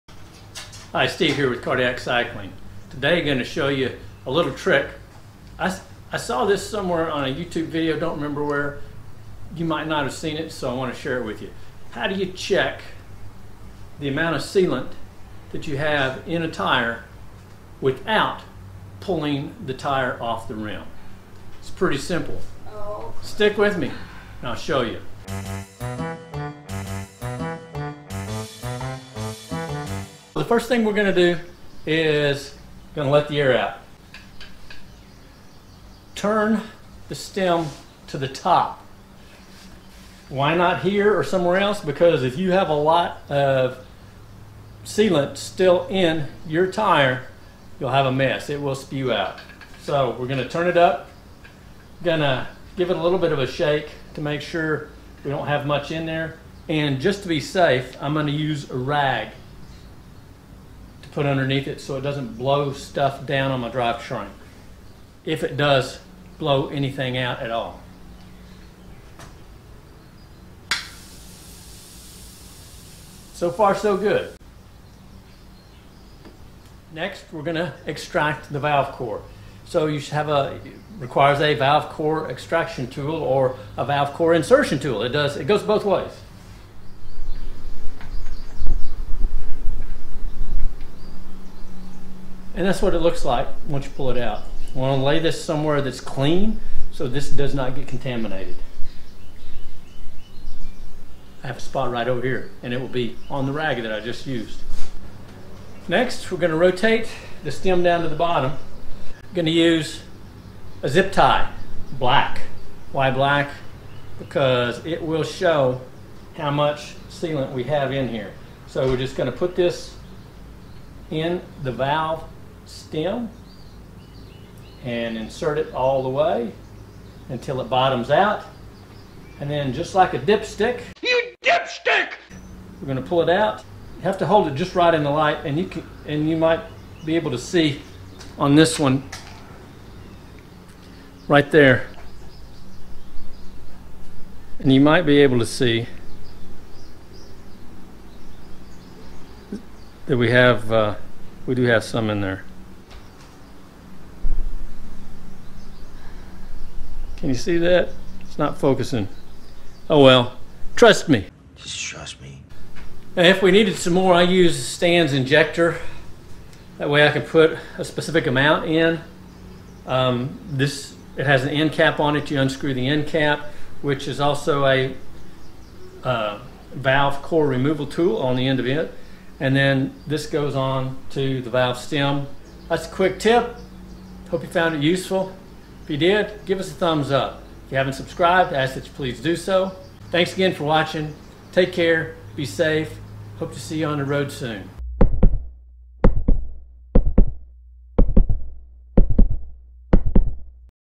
Tubeless Sealant Dip Stick
How do you know when it's time to add sealant to your tubeless tires?

You could take the wheel off your bike and shake it, listening for sloshing. OR, you could try this method which can be completed in less time than it takes to set the bike up on the repair stand!

You'll need a skinny zip tie and a valve core removal tool.

NOTE:  You need the tire elevated off the ground, as it is in a repair stand, to get a true read of the amount of sealant.  If the bike and tire on on the ground, in a "flat" state your reading will show LESS sealant than is actually present.

I received NOTHING from Stan's for this video;  it's a product I use myself and have had great experience with Stan's tire sealant.

Stans Sealant